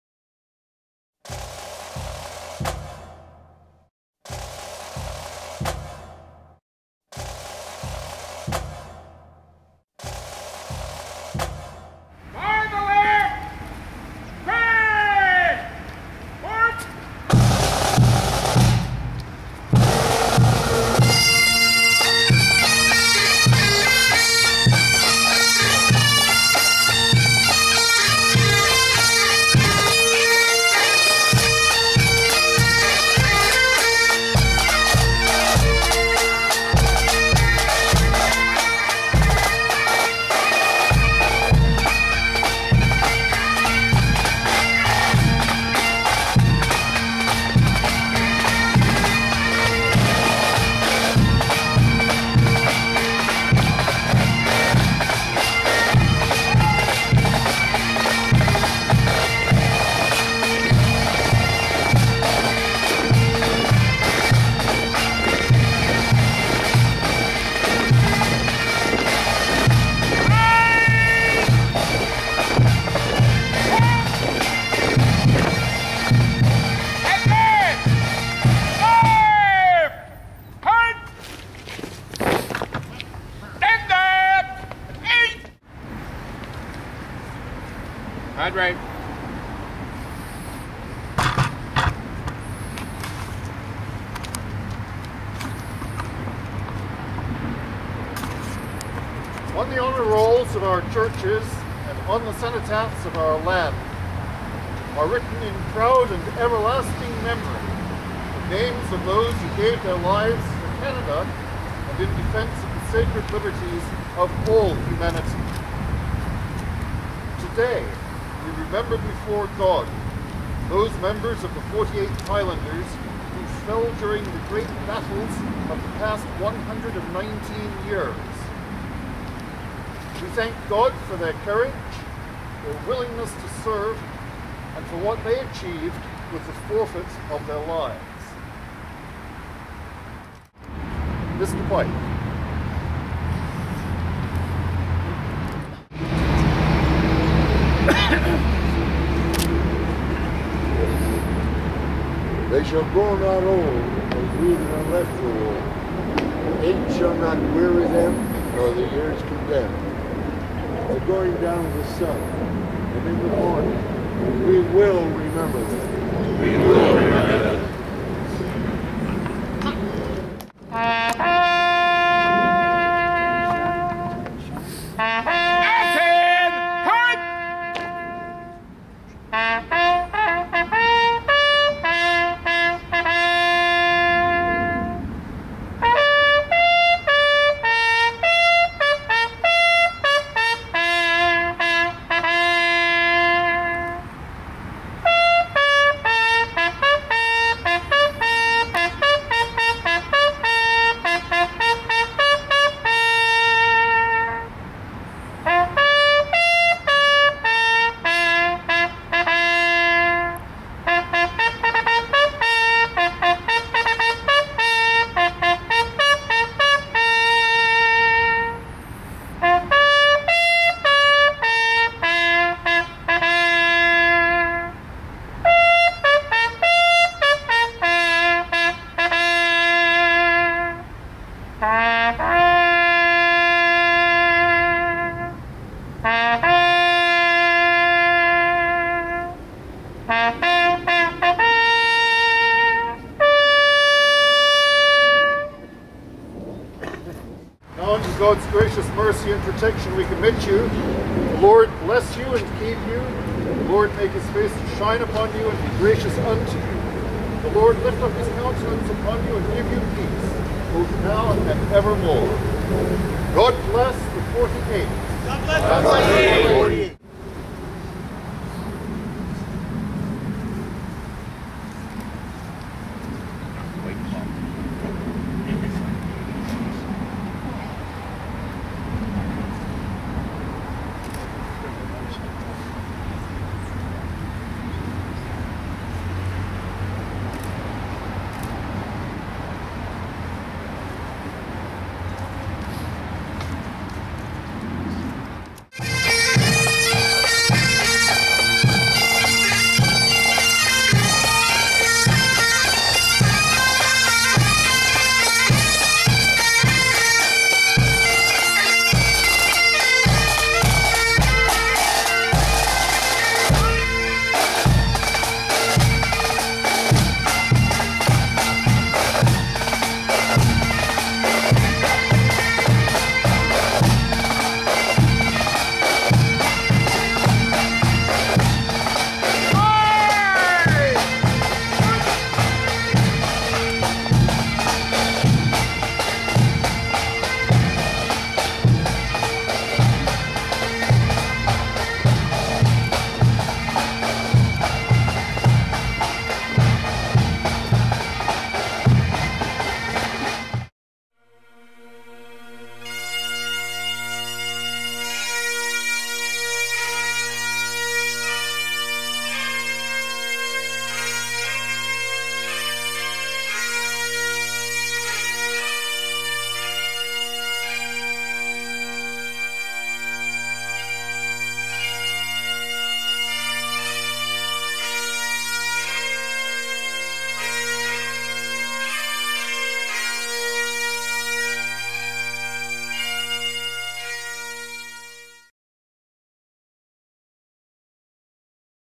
48th Highlanders of Canada Remembrance Day 2010 Mt. Pleasant Cemetery
Long live the traditions and customs of the 48th Highlanders of Canada.